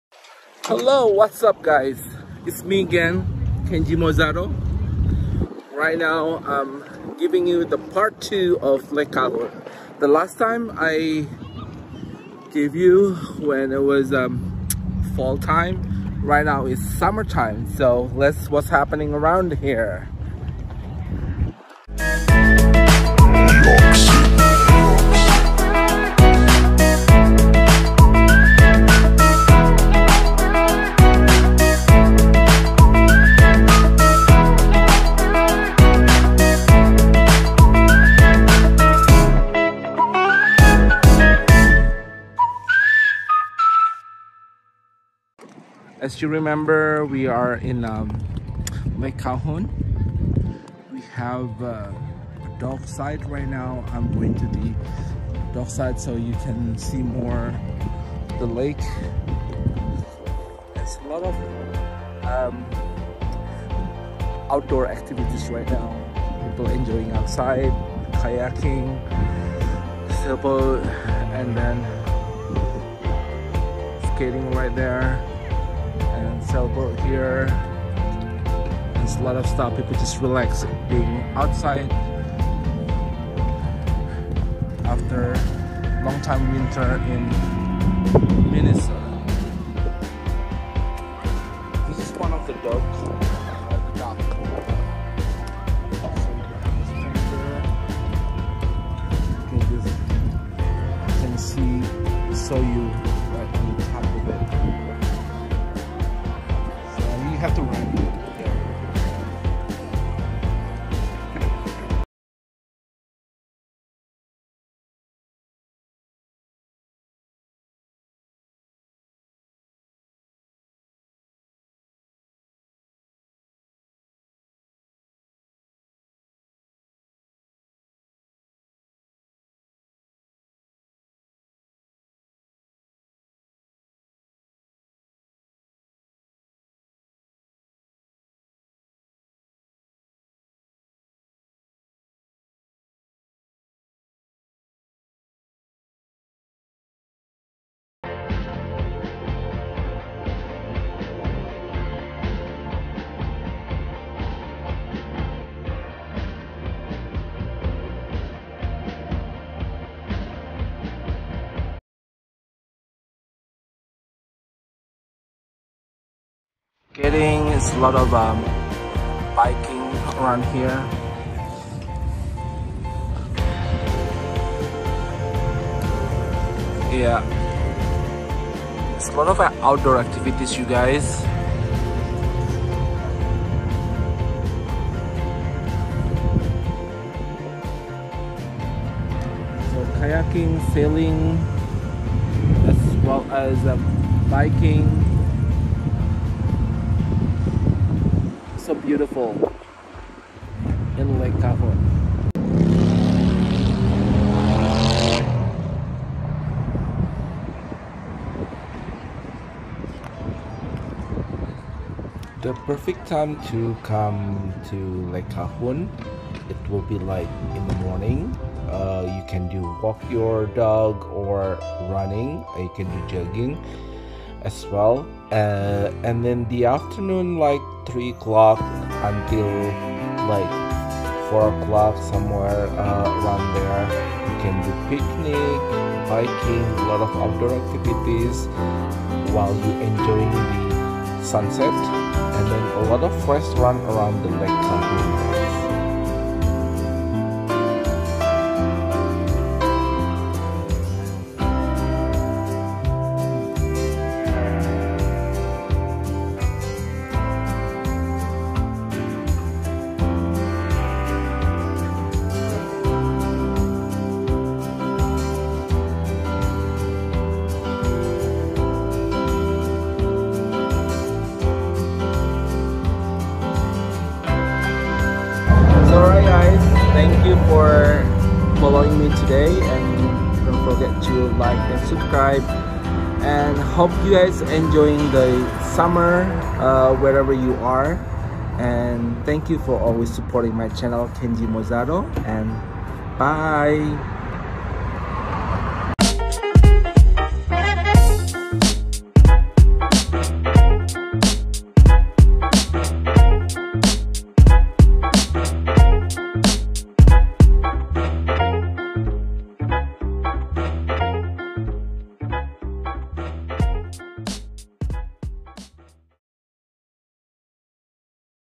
Beautiful Lake Calhoun (Ngabuburit di Minnesota) | Part 2 | Kenji Mozaro
Lake Calhoun (Ngabuburit di Minnesota) | Part 2 | Kenji Mozaro

Hey Guys!!
Month of Ramadhan so close in our heart while waiting for iftar, Im going to take you guys to Lake Calhoun today.more like activities during summer time, so stay tune guys!!

Thank you for check this out. Always look forward to your support.
Don’t forget to share your feedback in comment section. Thank You.